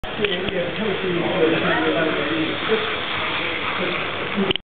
Faller Car System Kenworth truck
This Kenworth is one of the newly released items from faller for the Faller Car System, the truck consists of everything, so that means you can put any trailer or trailers with it.
More videos to come soon and picture slide shows to enjoy.